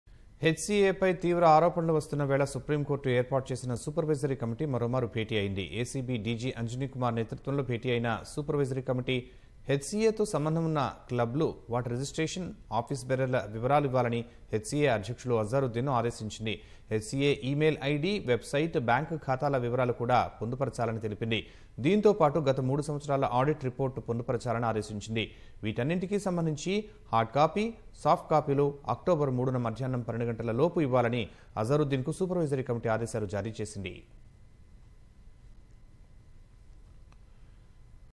HCA పై తీవ్ర ఆరోపణలు వస్తున్న వేళ మరోసారి భేటీ అయిన సుప్రీం కోర్టు ఏర్పాటు చేసిన సూపర్ వైజరీ కమిటీ
HCA పై తీవ్ర ఆరోపణలు వస్తున్న వేళ సుప్రీం కోర్టు ఏర్పాటు చేసిన సూపర్ వైజరీ కమిటీ మరోమారు భేటీ అయింది. ACB డీజీ అంజనీ కుమార్ నేతృత్వంలో భేటీ అయిన సూపర్ వైజరీ కమిటీ... HCA తో సంబంధం ఉన్న క్లబ్ లు వాటి రిజిస్ట్రేషన్,  ఆఫీస్ బేరర్ ల వివరాలు ఇవ్వాలని హెచ్ సీఏ అధ్యక్షులు అజారుద్దీన్ ను ఆదేశించింది.  HCA ఈమెయిల్ ఐడి,  వెబ్ సైట్, బ్యాంక్ ఖాతాల వివరాలు కూడా పొందుపరచాలని తెలిపింది. దీంతో పాటు గత 3సంవత్సరాల ఆడిట్ రిపోర్ట్ పొందుపరచాలని ఆదేశించింది. వీటన్నింటికీ సంబంధించి హర్డ్ కాపీ, సాప్ట్ కాపీలు అక్టోబర్ 3న మధ్యాహ్నం 12 గంటల లోపు ఇవ్వాలని... అజారుద్దీన్ కు సూపర్ వైజరీ కమిటీ ఆదేశాలు జారీ చేసింది-